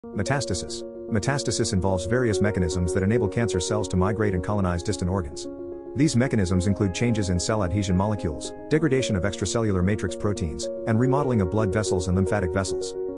Metastasis simplified
1. Definition and Process: Metastasis refers to the spread of cancer cells from the primary tumor to distant sites in the body. It is a complex multistep process involving several biological events. Cancer cells break away from the primary tumor, invade nearby tissues, enter the bloodstream or lymphatic system, travel to distant organs or tissues, and establish secondary tumors at those sites. Metastasis is a major factor contributing to the progression and severity of cancer.

2. Mechanisms of Metastasis: Metastasis involves various mechanisms that enable cancer cells to migrate and colonize distant organs. These mechanisms include changes in cell adhesion molecules, degradation of extracellular matrix proteins, and remodeling of blood vessels and lymphatic vessels. Cancer cells may also undergo epithelial-mesenchymal transition (EMT), where they acquire characteristics that enhance their ability to invade and metastasize. Additionally, interactions between cancer cells and the microenvironment of distant organs play a role in facilitating metastasis.

3. Clinical Implications: Metastasis significantly affects cancer prognosis and treatment. The spread of cancer to distant sites can make it more challenging to treat effectively. Once cancer has metastasized, it is often considered advanced or stage IV. Treatment options may include systemic therapies such as chemotherapy, targeted therapy, immunotherapy, or radiation therapy, depending on the specific characteristics of the cancer. Understanding the mechanisms and pathways involved in metastasis is crucial for developing targeted therapies to inhibit or prevent the spread of cancer cells.